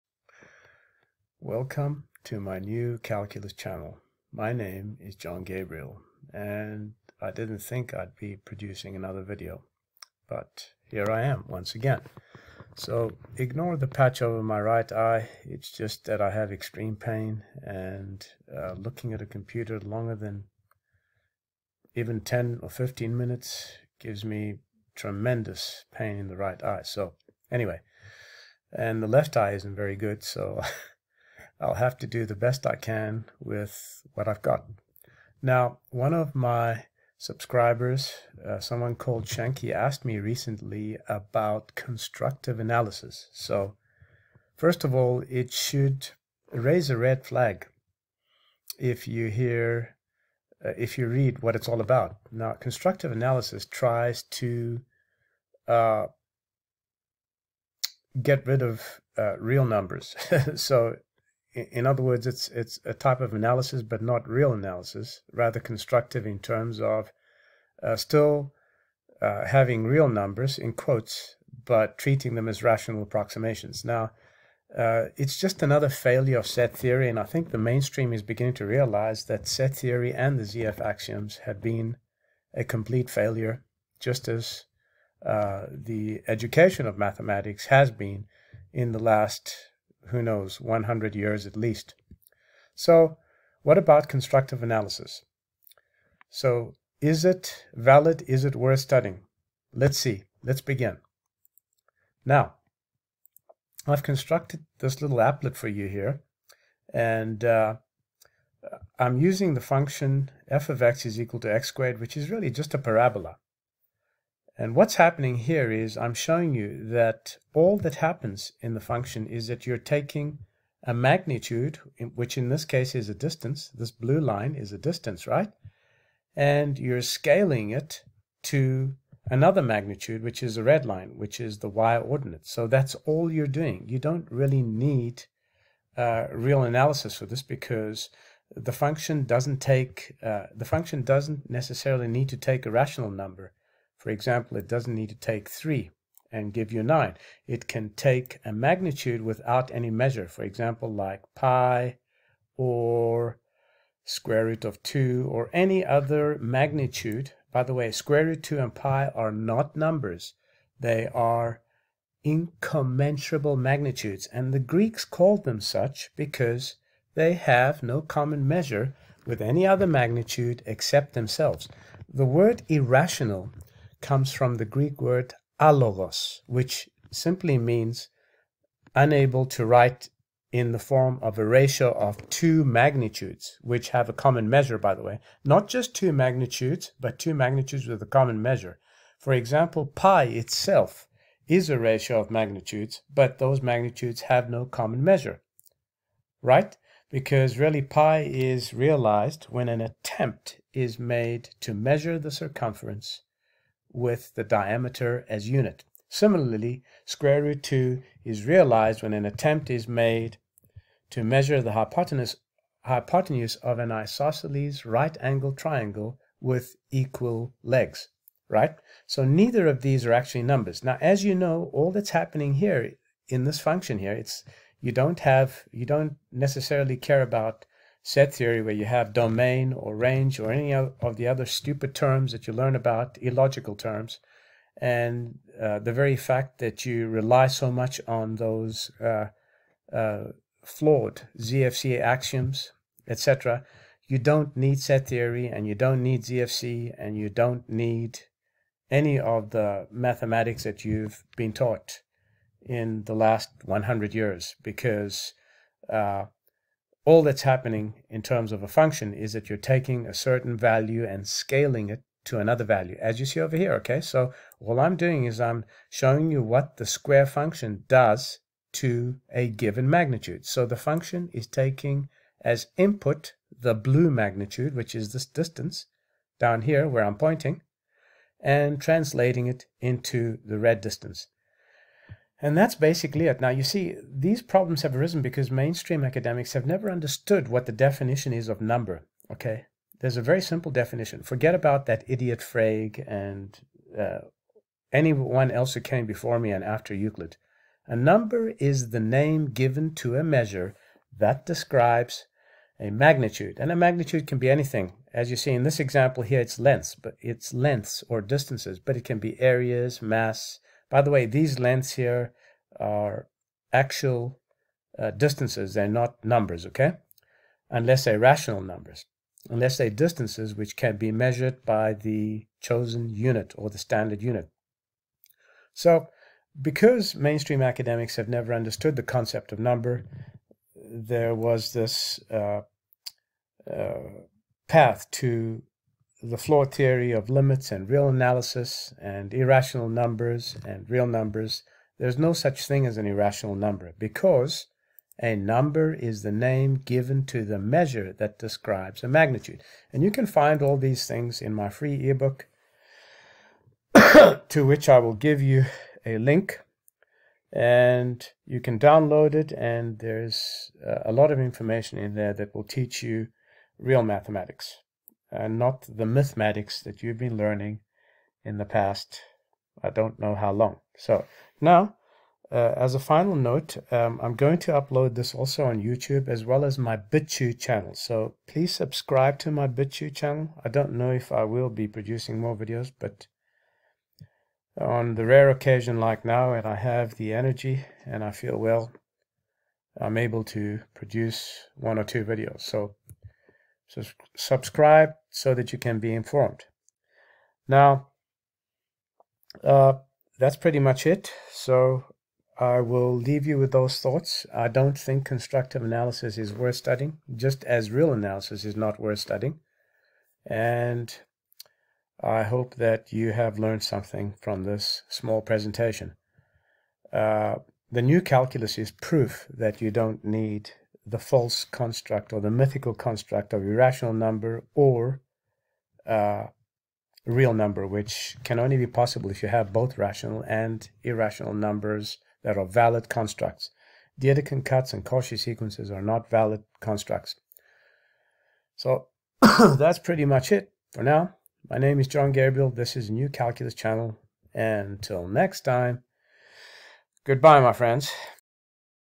Is Constructive Analysis worth studying?
Constructive analysis is an excuse for real analysis which is a flawed topic based on an object that doesn't exist - the "real number".

There is no need for such nonsense and the New Calculus proves neither such analysis is required: